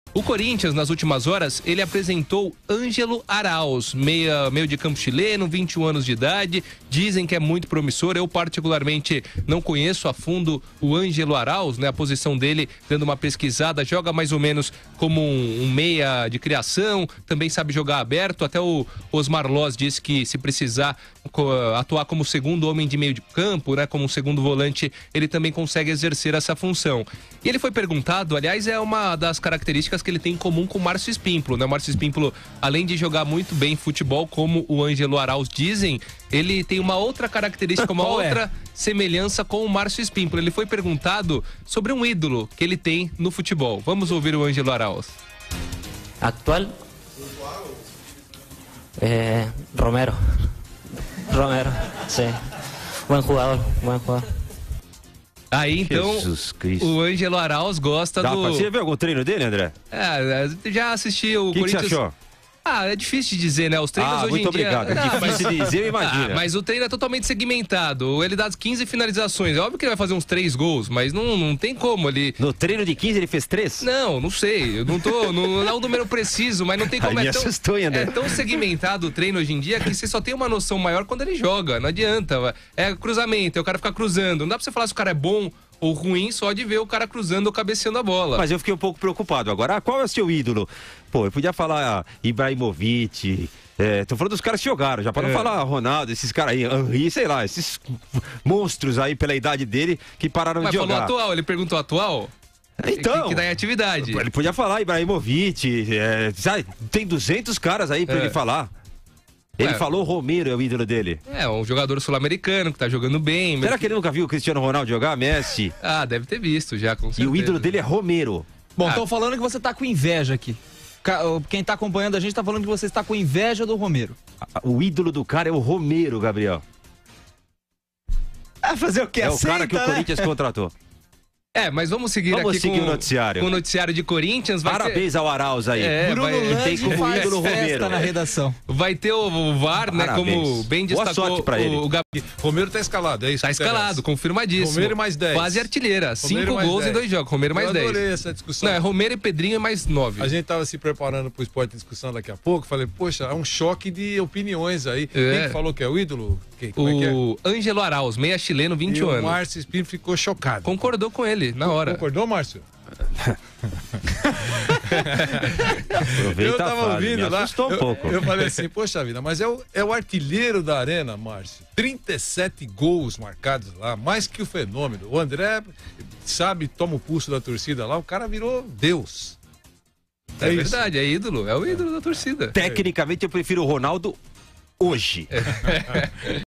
Araos exalta Romero, e Spimpolo IRONIZA: "não conhece CR7?"
Marcio Spimpolo ironizou Ángelo Araos, reforço do Corinthians que disse que o seu ídolo no futebol era Ángel Romero.